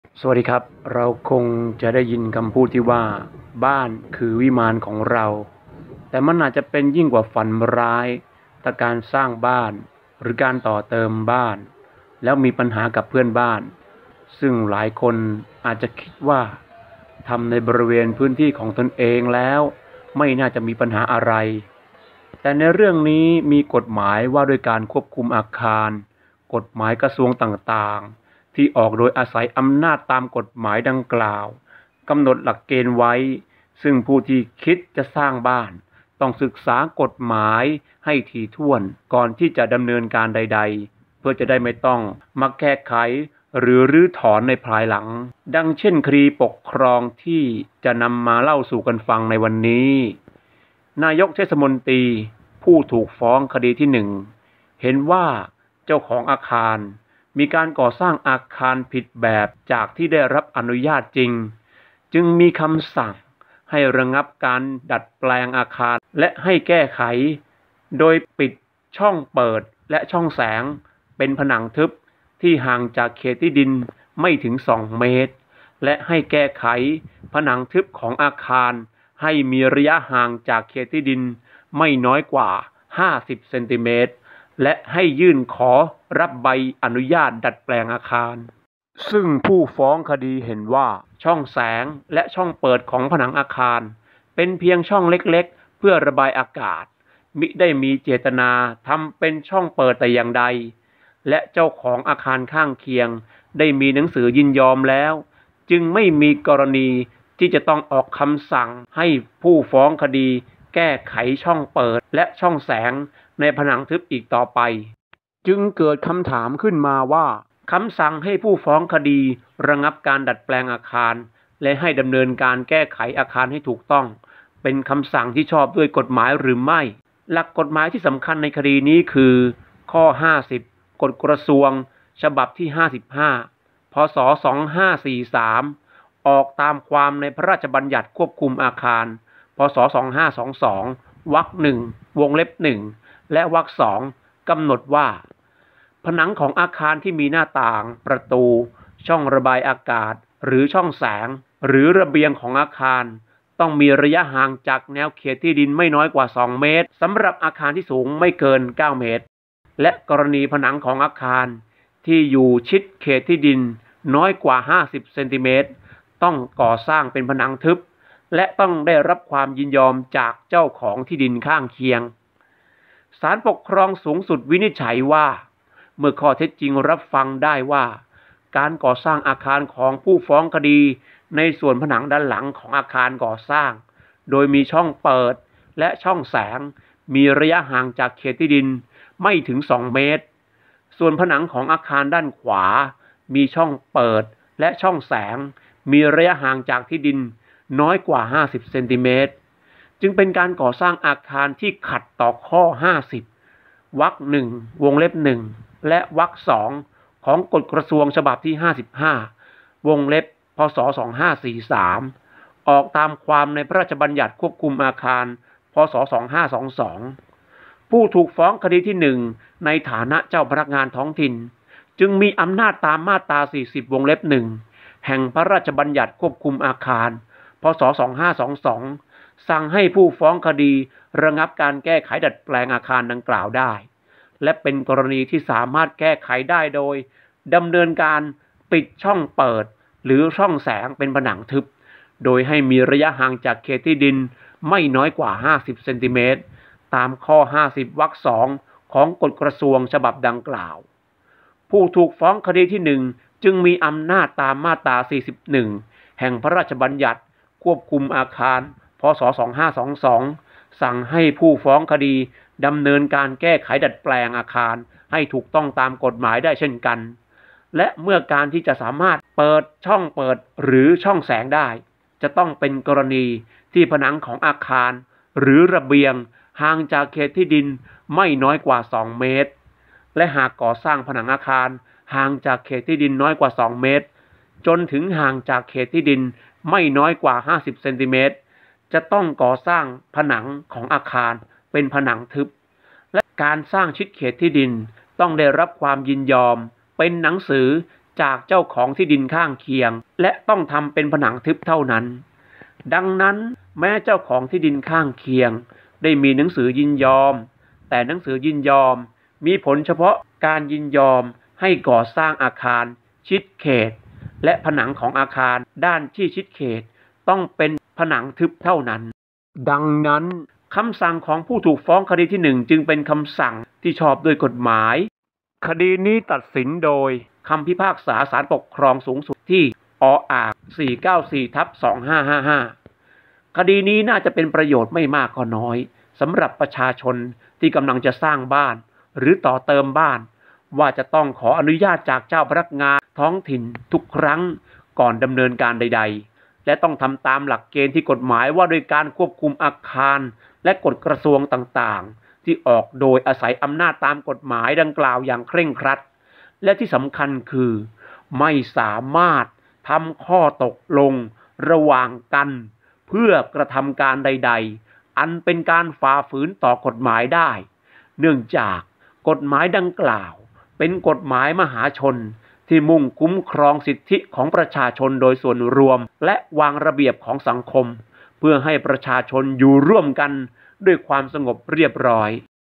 คนปลูกบ้านต้องเข้าใจ ไม่สามารถทำข้อตกลงระหว่างกันเพื่อกระทำการใดๆอันเป็นการฝ่าฝืนต่อกฎหมายได้
หวังว่าคลิปนี้คงจะเป็นประโยชน์ต่อผู้ชมไม่มากก็น้อยครับ หากผิดพลาดประการใดได้โปรดชี้แนะด้วยครับ @teukthamrai